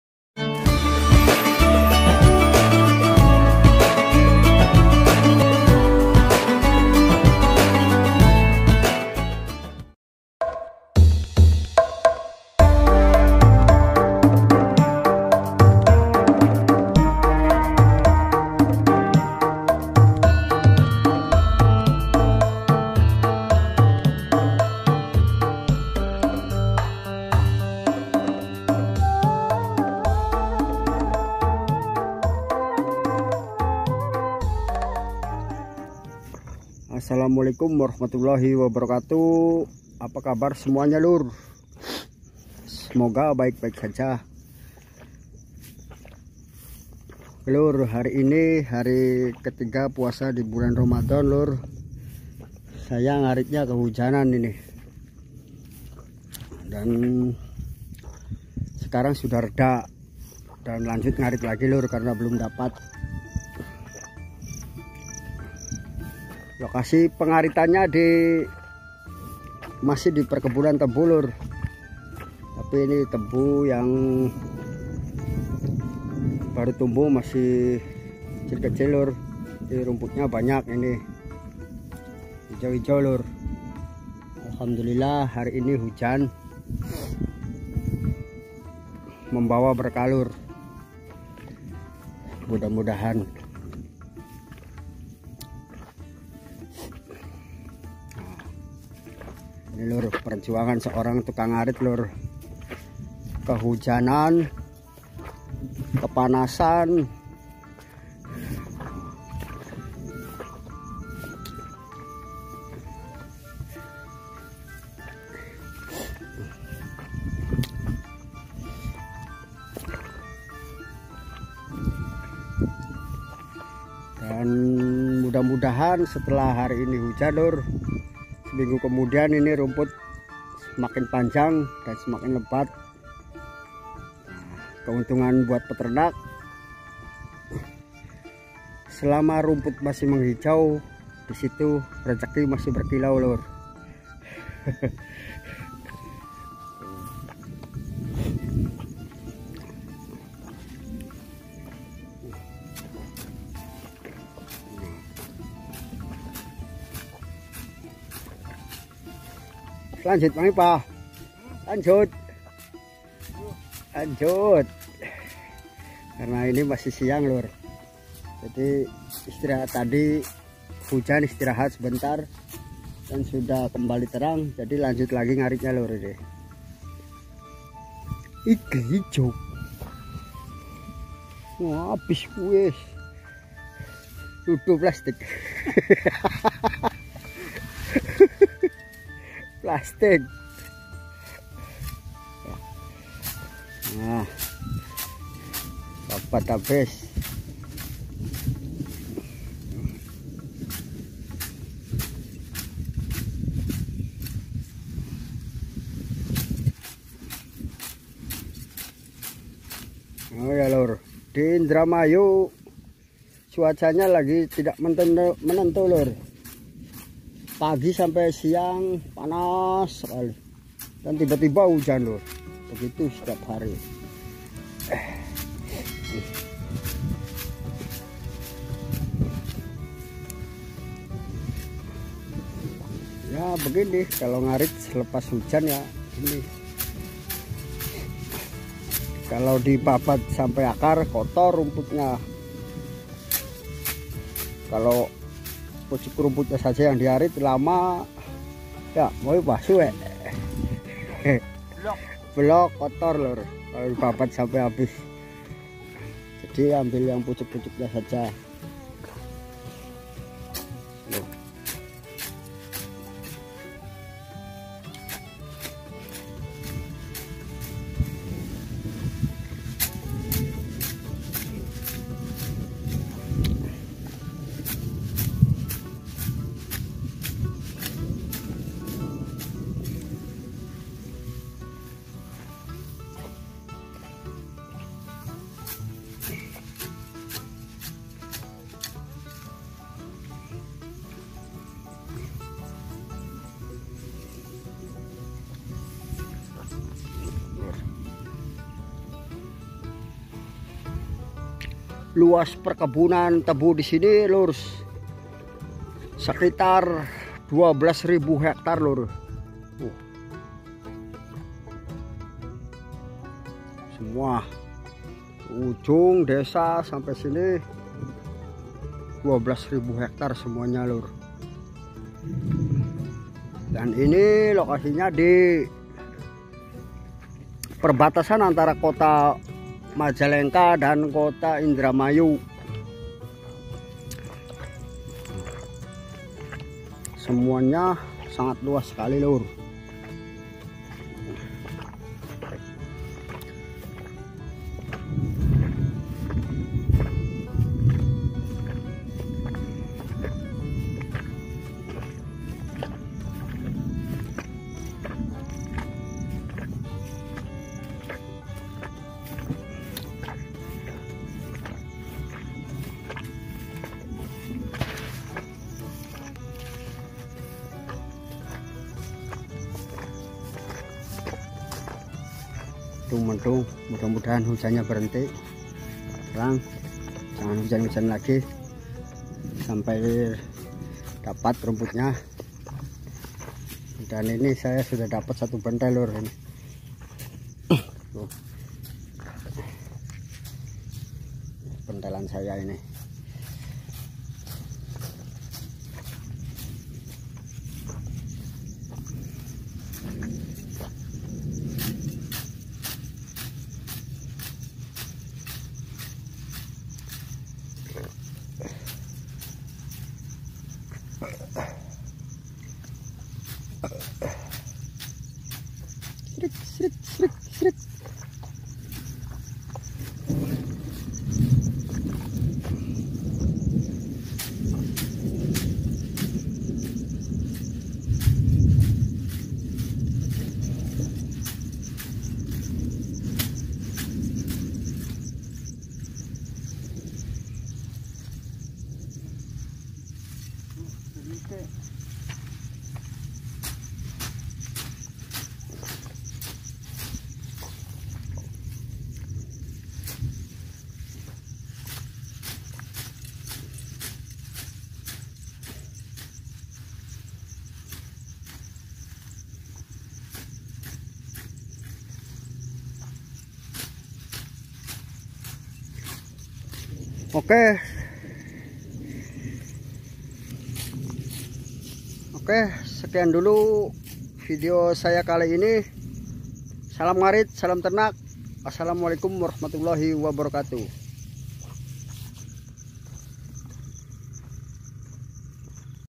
NGARIT SUKET HARI KE TIGA PUASA RAMADHAN DI GUYUR HUJAN
kegiatan sehari hari ngarit suket /rumput untuk pakan ternak domba kambing..di hari ke tiga puasa ramdhan di guyur hujan luur...